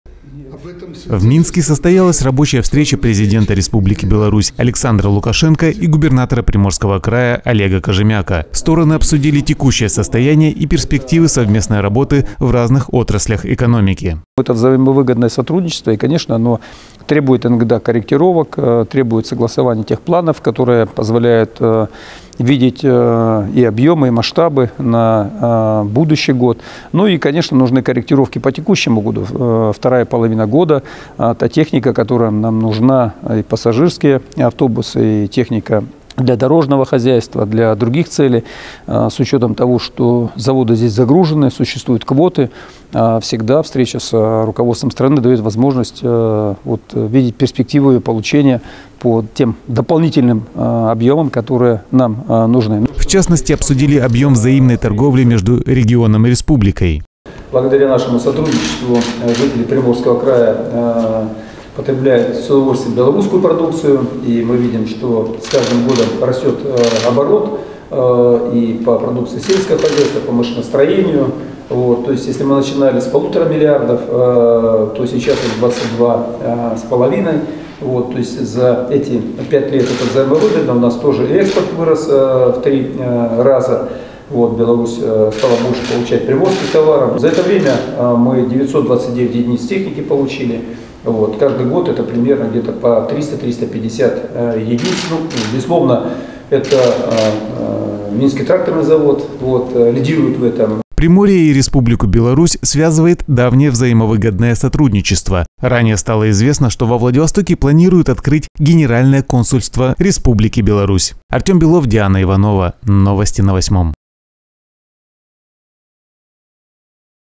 Прошла рабочая встреча Губернатора Приморья с Президентом Республики Беларусь.
В Минске состоялась рабочая встреча Президента Республики Беларусь Александра Лукашенко и Губернатора Приморского края Олега Кожемяко. Стороны обсудили текущее состояние и перспективы совместной работы в разных отраслях экономики.